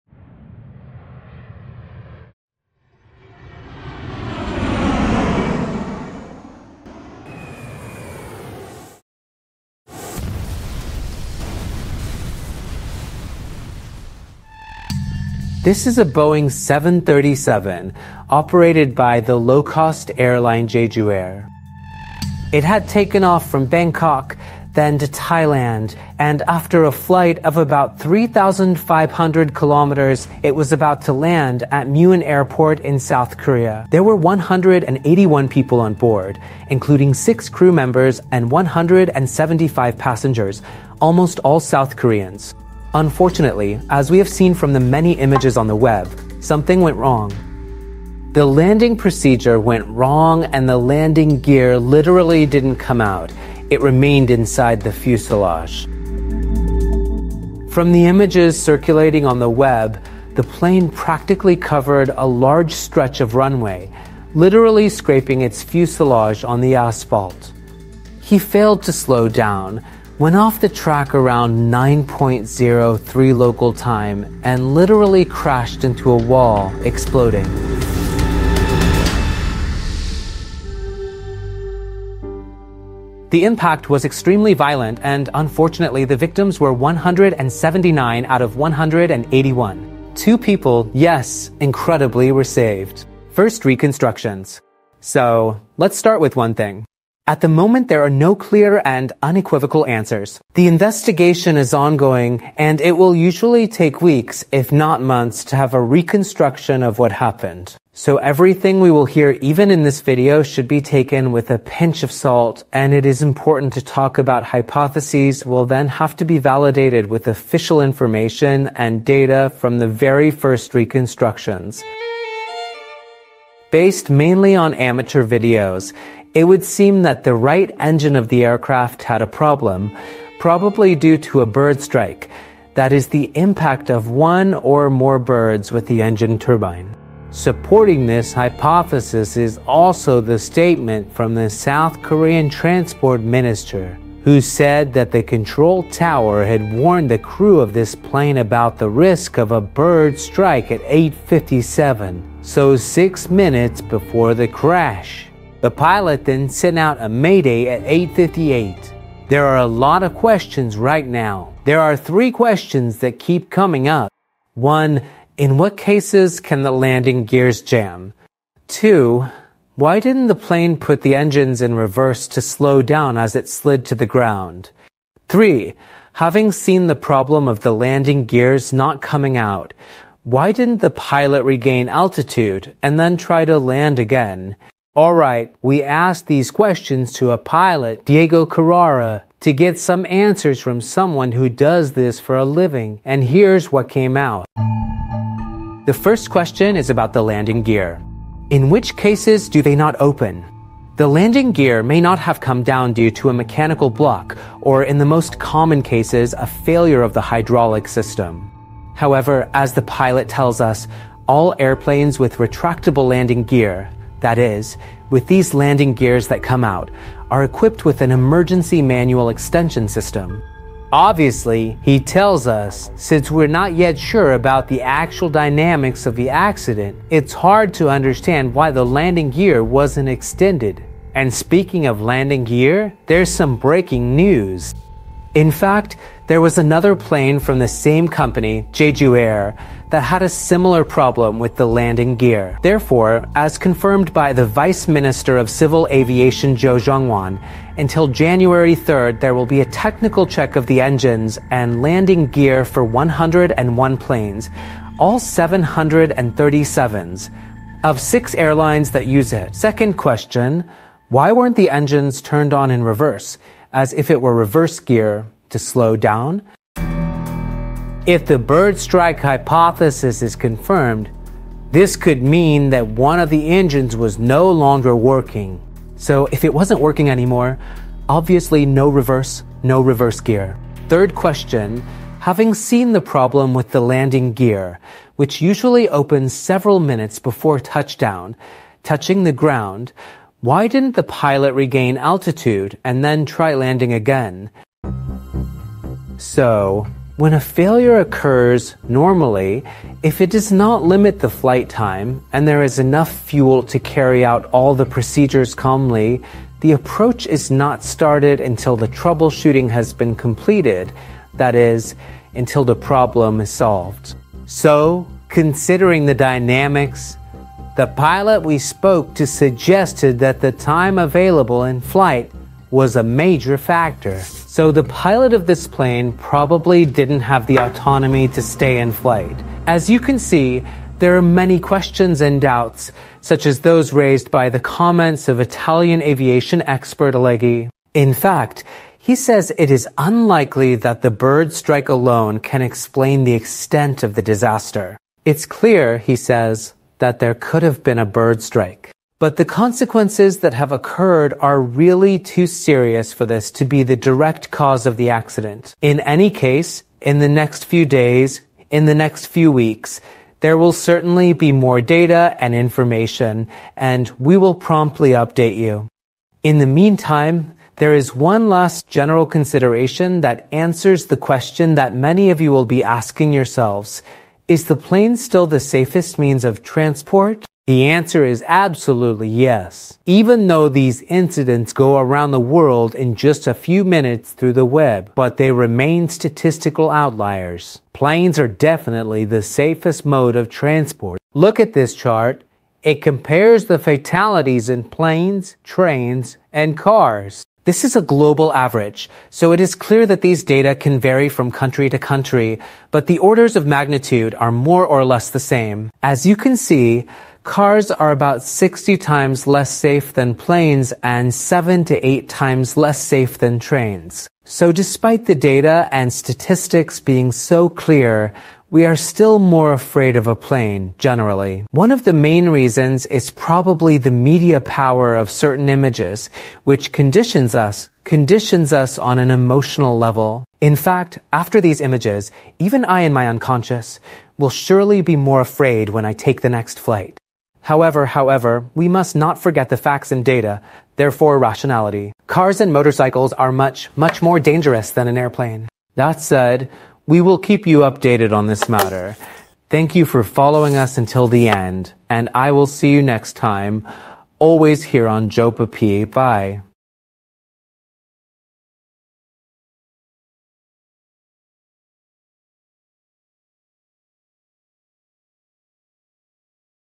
Plane Crash in South Korea with Unseen 3D Animations - Hypotheses, Causes and Questions
In South Korea, a plane crash involved a Boeing 737-8AS of Jeju Air, which went off the runway during landing in Muan and caused 179 victims out of 181 passengers on board. But what happened? And why did the landing gear not work? In this video, through unpublished 3D animations, we analyze the dynamics of the accident, from the first reconstructions and the hypothesis of a possible bird strike, to the technical problems that could have aggravated the situation.

00:00 South Korea plane crash: what happened
02:07 First hypotheses and reconstructions: the ongoing investigations
03:42 Key questions answered by an expert pilot
04:19 Why didn't the plane's landing gear work?
05:24 Why weren't the engines reversed to slow down?
05:47 Why didn't the pilot regain altitude?
07:22 Is the plane still the safest means of transport?